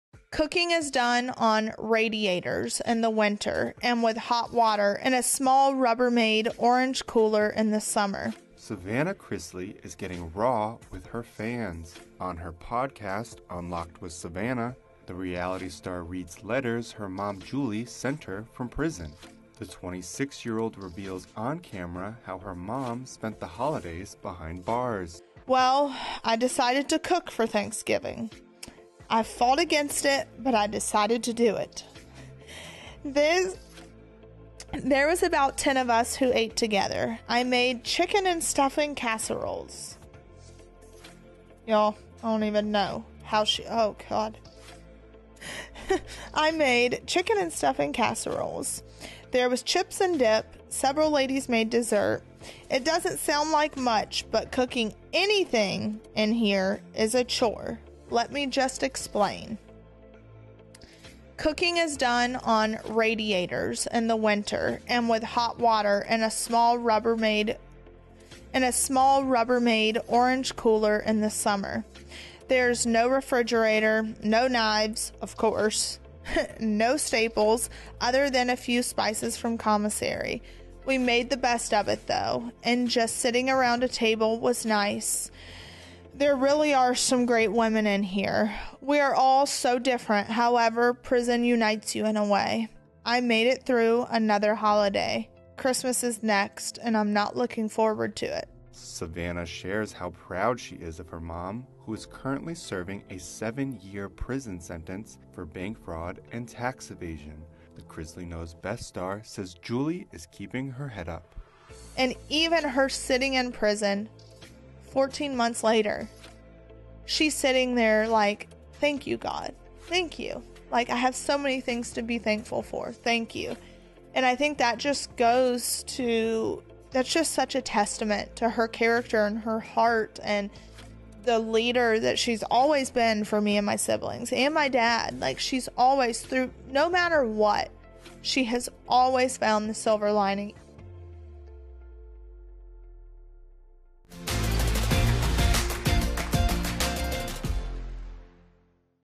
You Won't Believe How Julie Chrisley Made a Holiday Casserole IN PRISON | E! News
Julie Chrisley got inventive when she made a holiday meal for her fellow prison inmates. Find out exactly how the Chrisley Knows Best alum whipped up a crafty casserole behind bars.